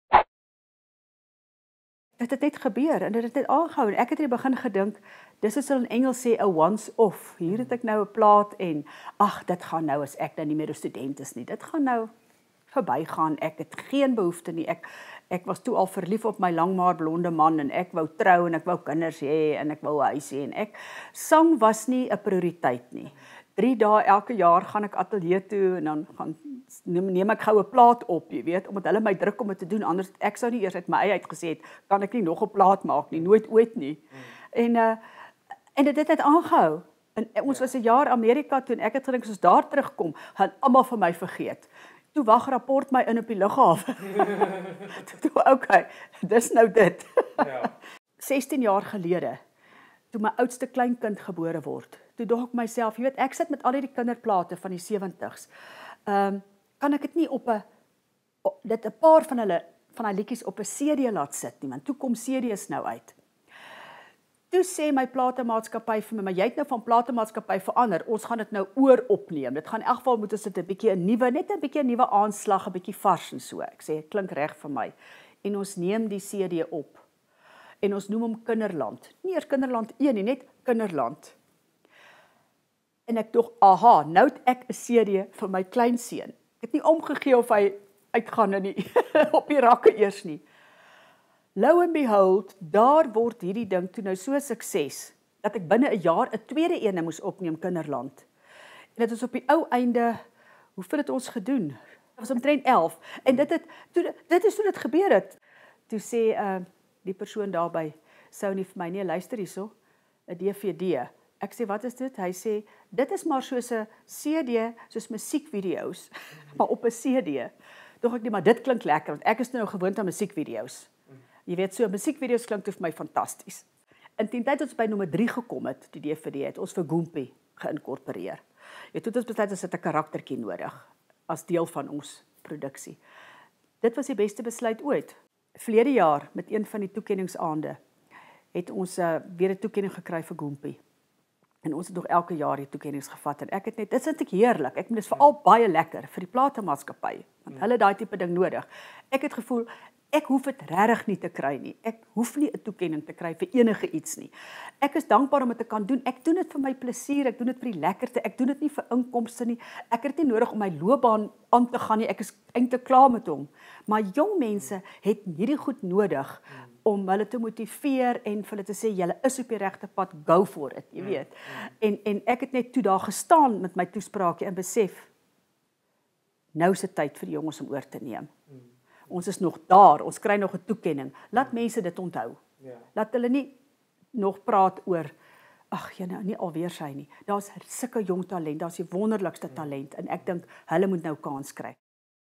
Carike se stokperdjie duur 50 jaar
Carike Keuzenkamp vertel dat sang nie vir haar 'n prioriteit was nie. Sy het gedink dit is sommer so 'n stokperdjie, min het sy geweet dat sy 'n sangloopbaan van 50 jaar gaan vier.